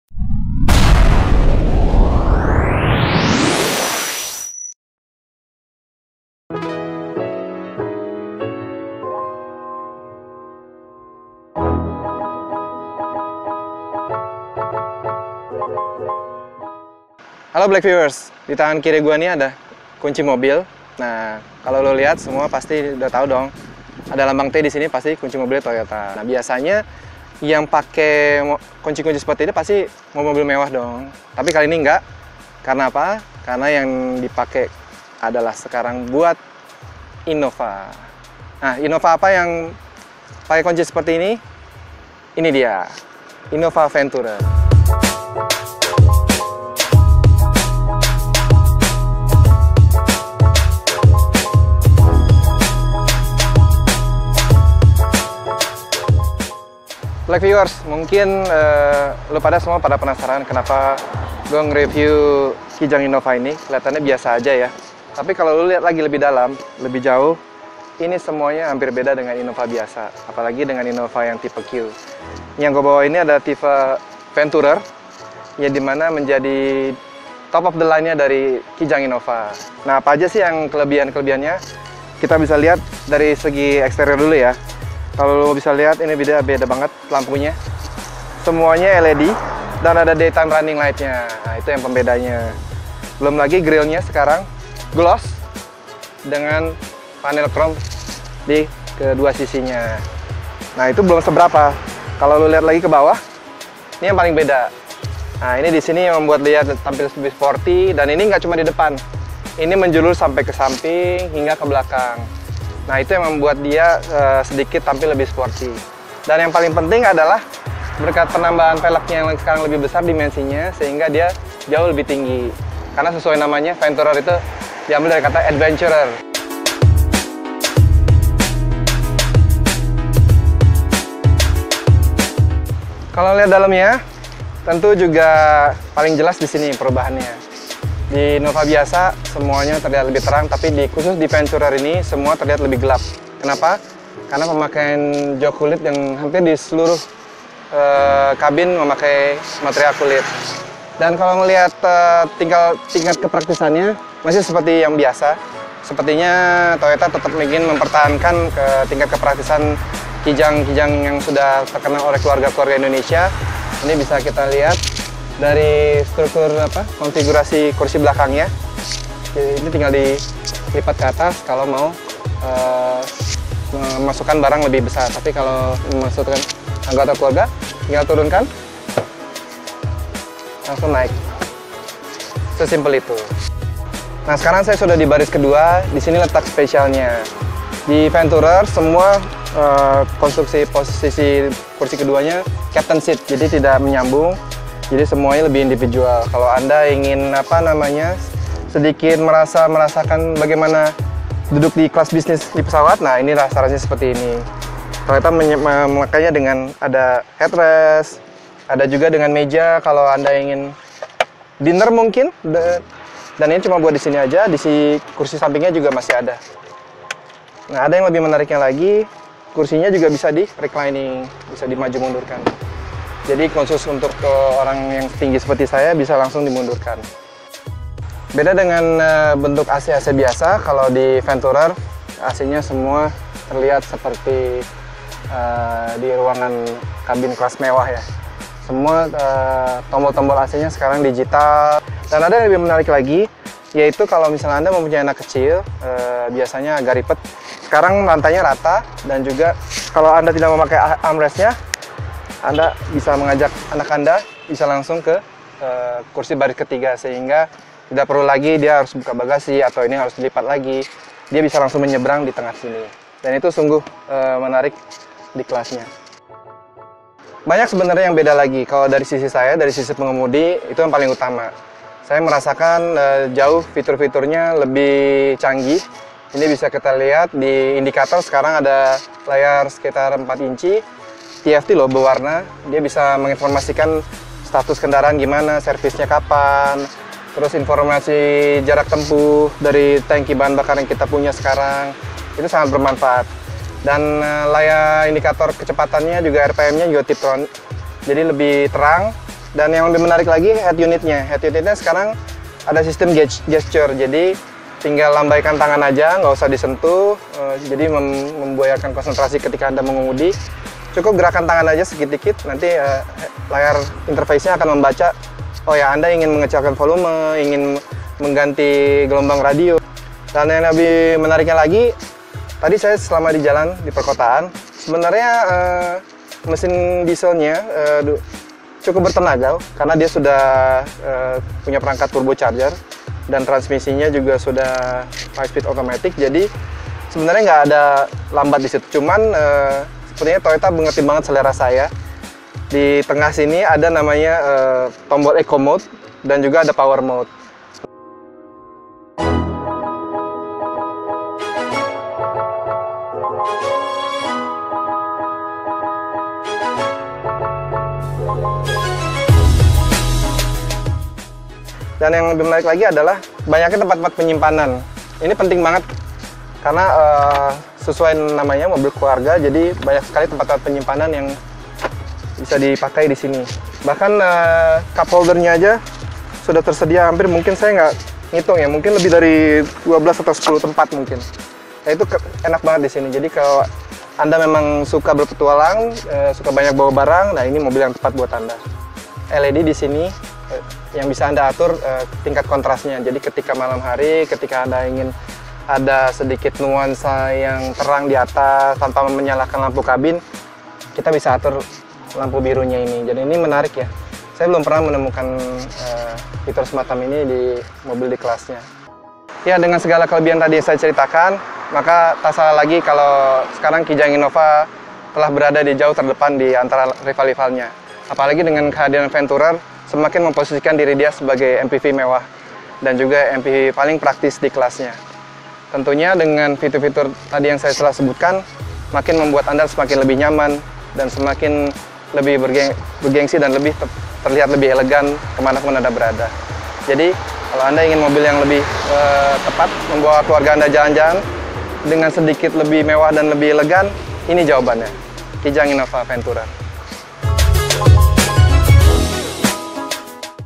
Toyota Kijang Innova Venturer -Practicality Meets Exclusivity-
Videografer : Andy Kuswandi

Practicality Meets Exclusivity
Dalam kurun waktu lebih dari 30 tahun, Toyota Kijang tak hanya memenuhi jalan-jalan di Indonesia namun juga menghiasi hati keluarga Indonesia. Jumlah penjualannya di dalam negeri fenomenal dan belum tersaingi. Bahkan pemiliknya menganggap kendaraan ini sebagai investasi keluarga. Tidak ada yang bisa menyaingi harga jual kembali kendaraan ini di pasar dalam negeri. Tak heran depresiasinya tak begitu drastis turunnya.